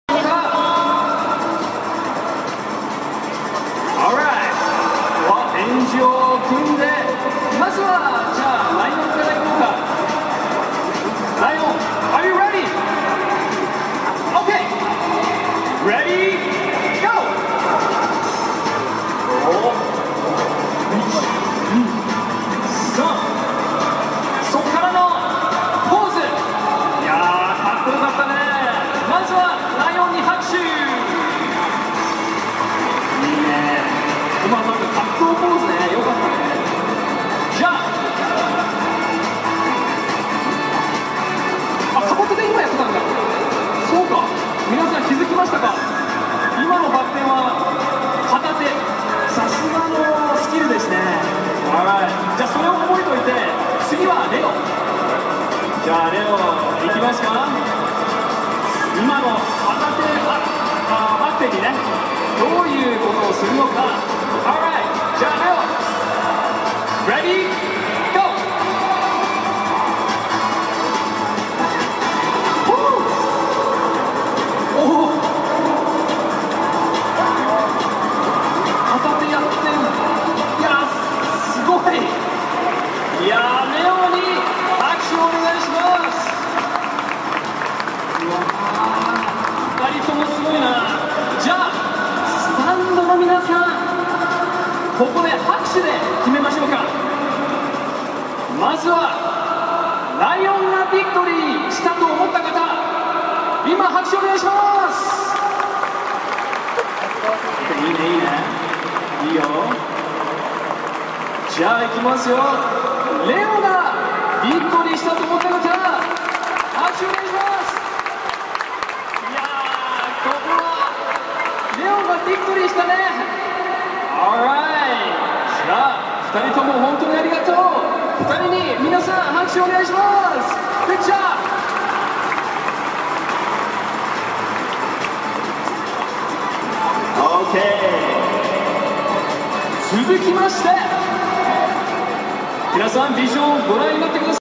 2017.7.27 統一ライオンズのマスコット「ライオン」と埼玉西武ライオンズのマスコット「レオ」のバク転対決
2017年7月27日 埼玉西武ライオンズvsオリックス・バファローズ メットライフドーム